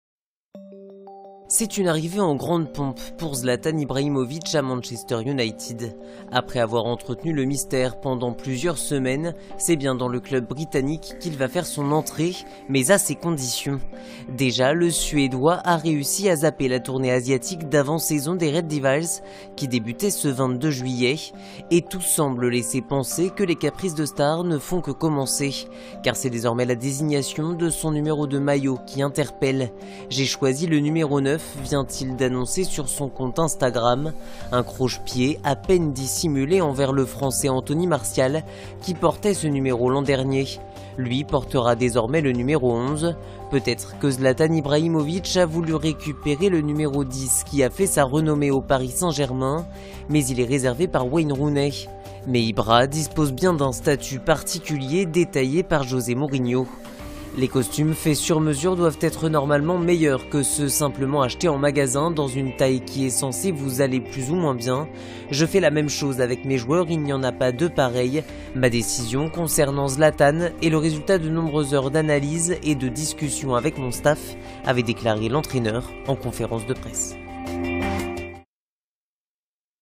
Manchester United : le coup bas de Zlatan Ibrahimovic à Anthony Martial
C’est une arrivée en grandes pompes pour Zlatan Ibrahimovic à Manchester United. Après avoir entretenu le mystère pendant plusieurs semaines, c’est bien dans le club britannique qu’il va faire son entrée, mais à ses conditions.